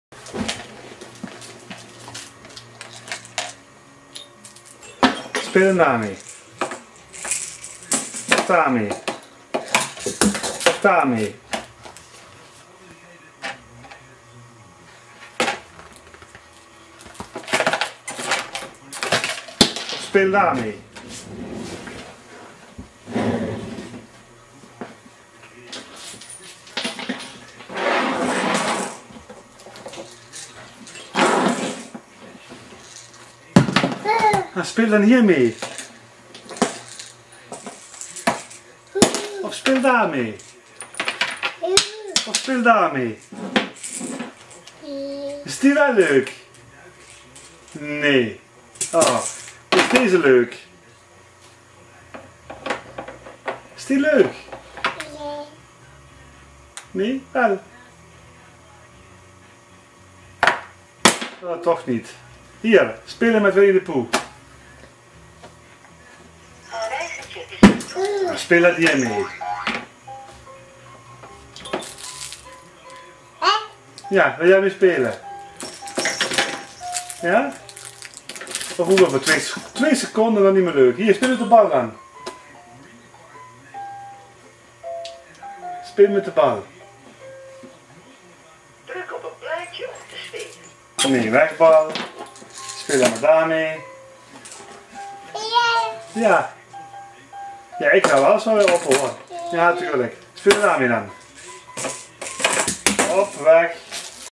Silas doens't want to play
Little Silas is annoyed with all his toys, he throws away every toy.